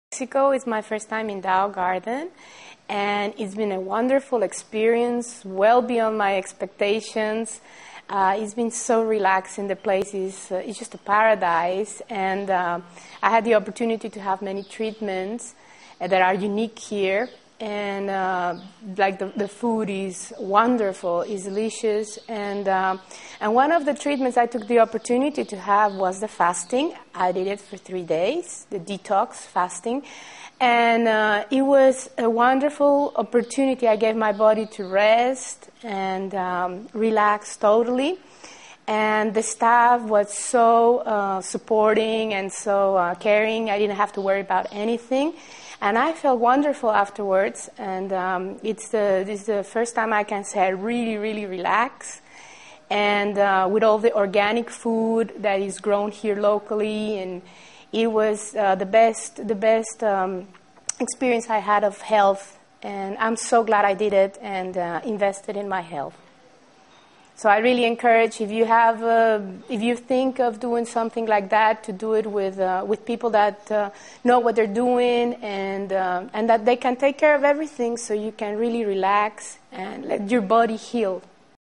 Mantak Chia # Customer Fasting Interview 2009 at Tao Garden with Ms. Liliana Gonzalez
Master Chia's Training Center at Tao Garden Health Spa & Resort, Chiangmai, Thailand